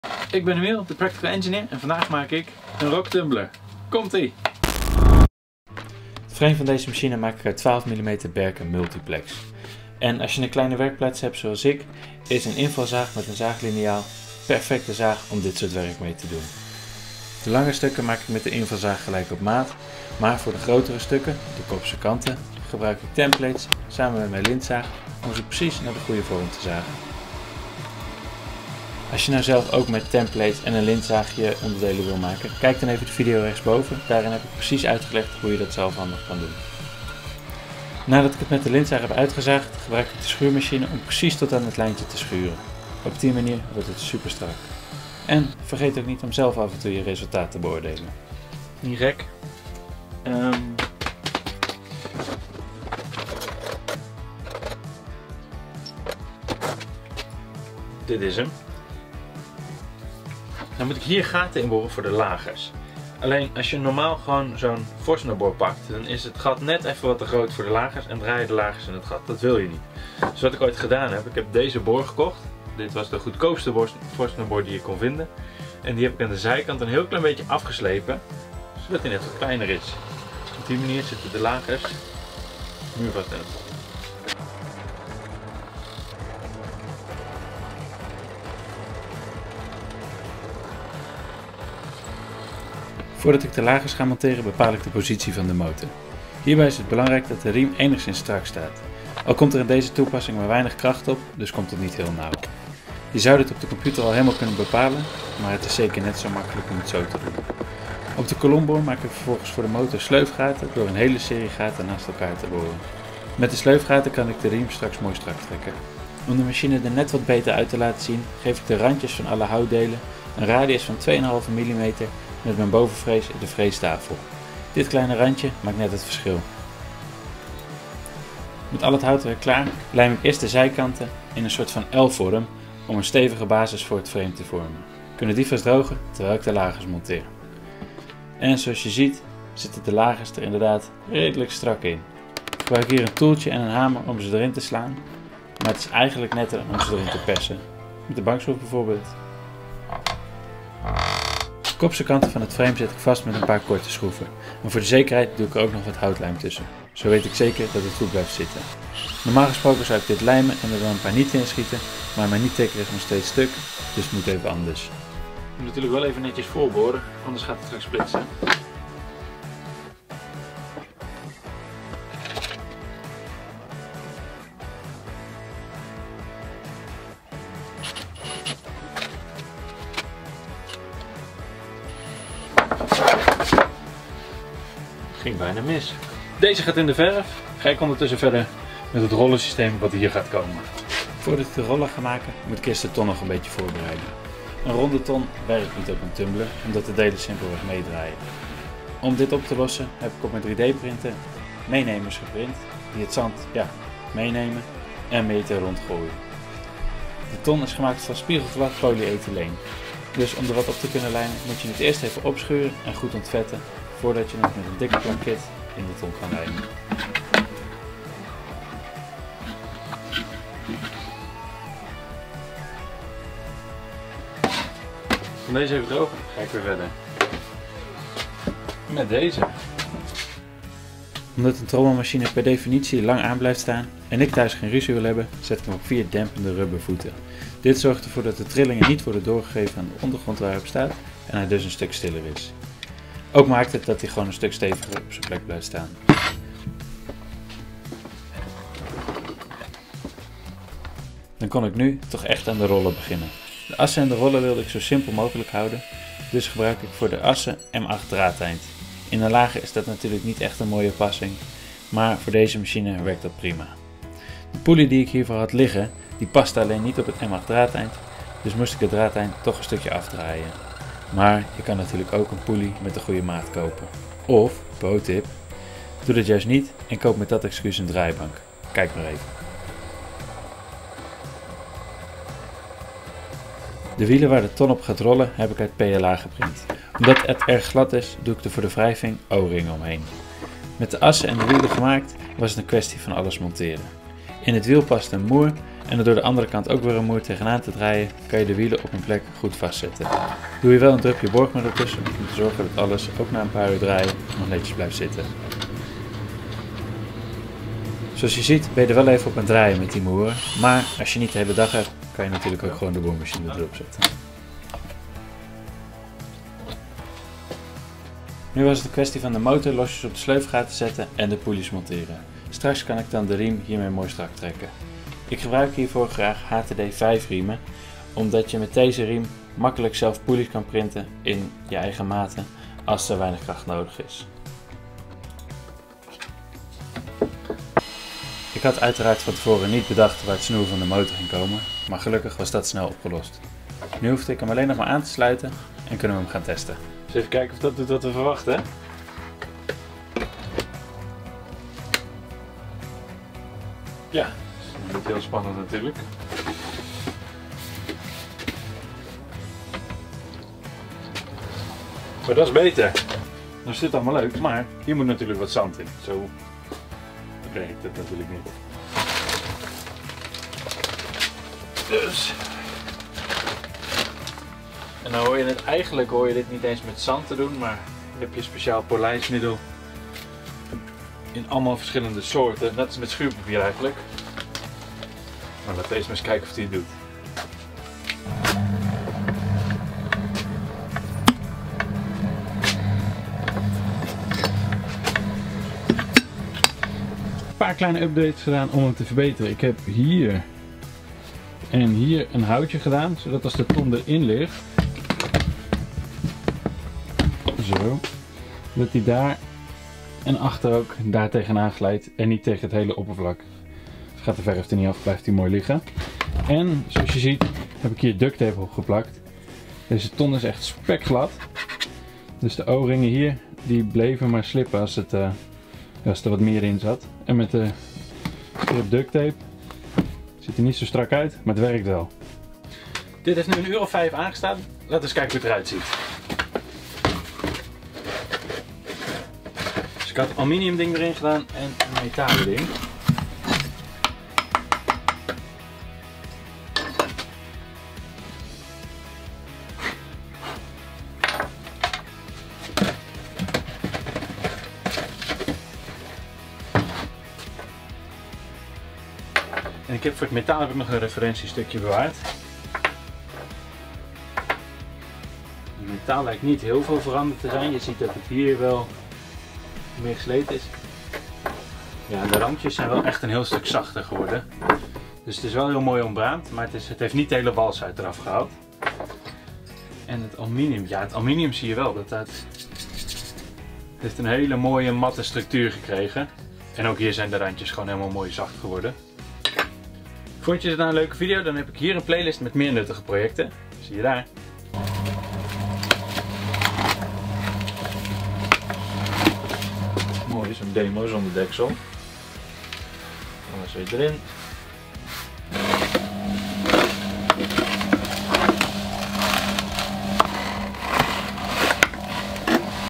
How to make a rock tumbler in a day
A rock tumbler is a machine that can easily polish your 3d printed or metal parts. I wanted to have one for a long time and it was finally time to make one.